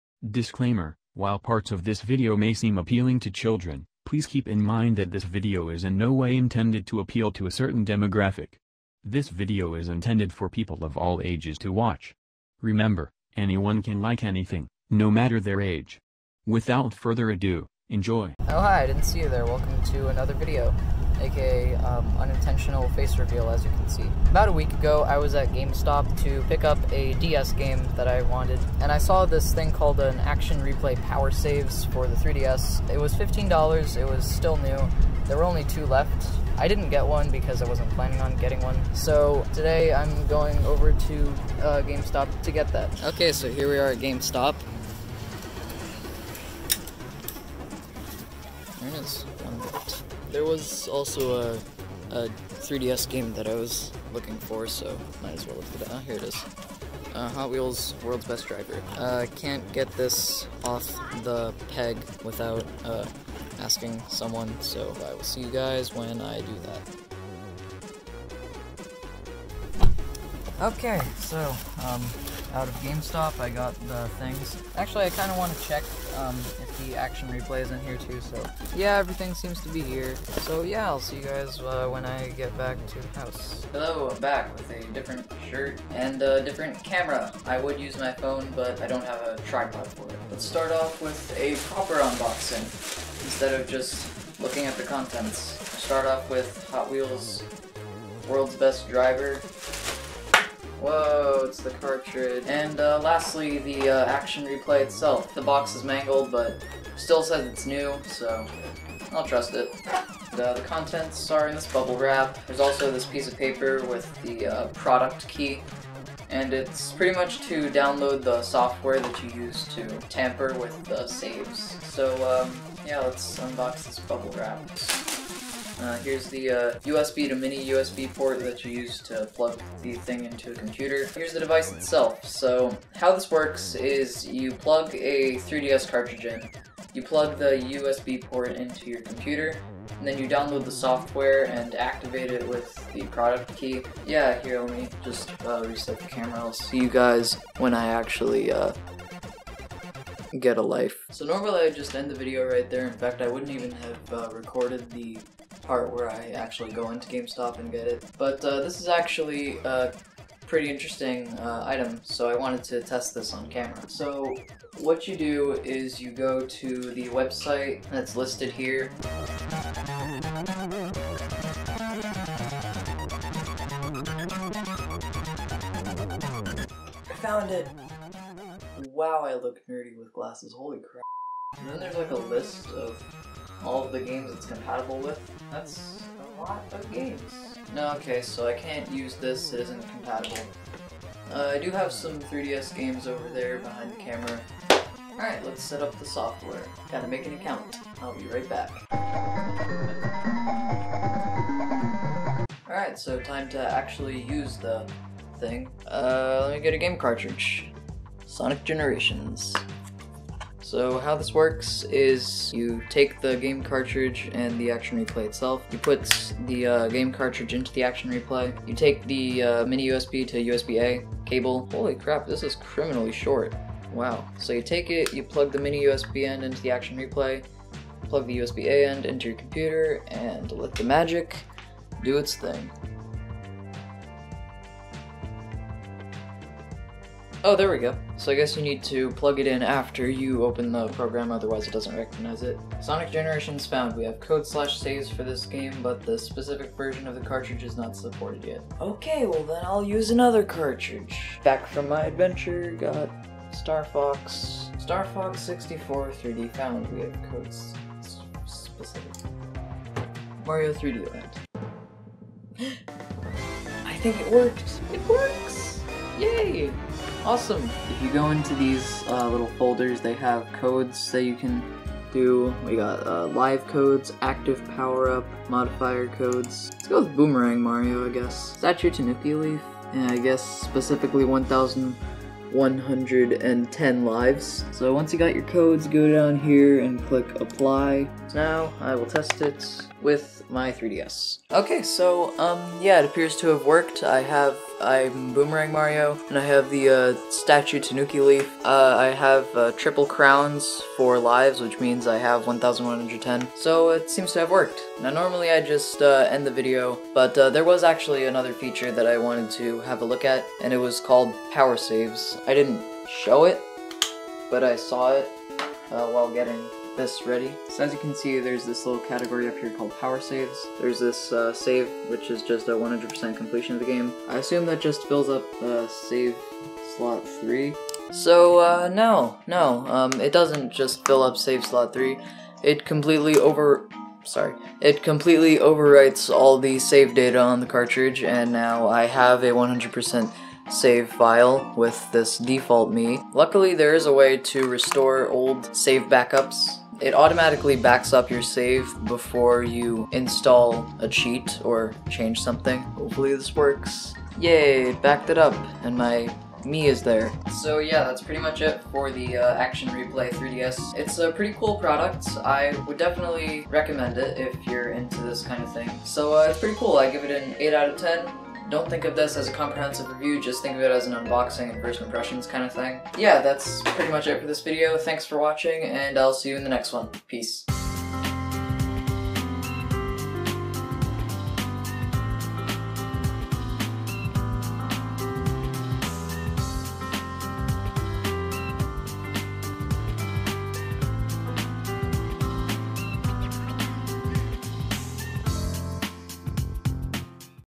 3DS ACTION REPLAY!
I set out on an adventure to get a 3DS Action Replay from GameStop! Was it worth it? Let's find out!

Nintendo Switch Friend Code: SW-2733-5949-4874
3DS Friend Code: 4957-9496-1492
PSN ID: LeqendOP